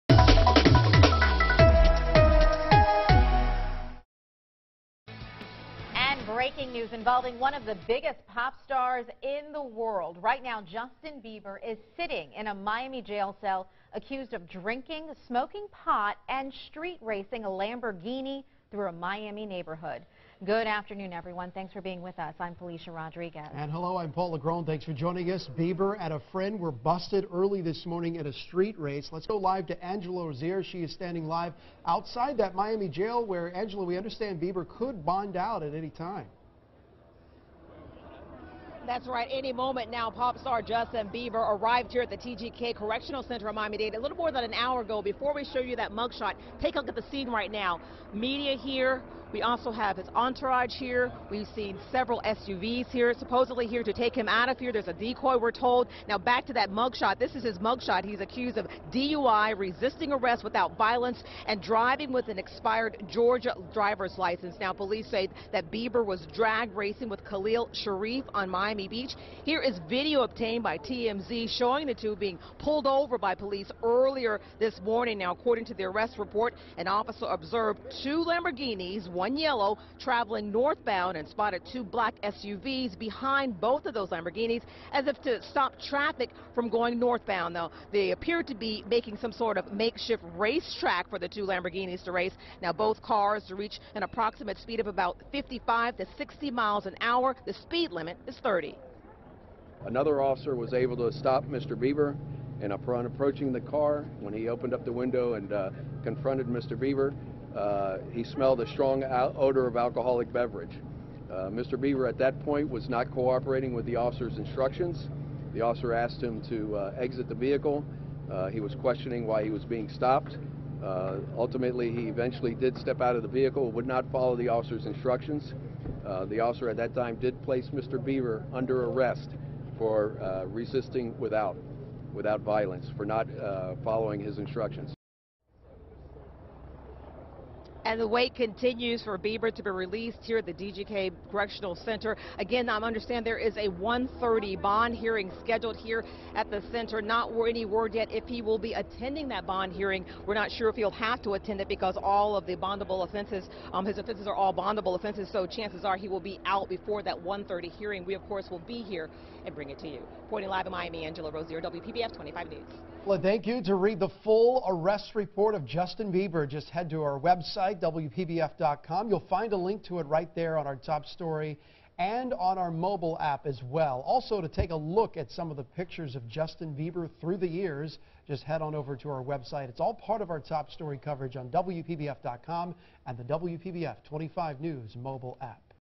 Police spokesperson says Bieber 'was not cooperating'
A police official in Miami Beach describes the circumstances surrounding Justin Bieber's DUI arrest early Thursday morning.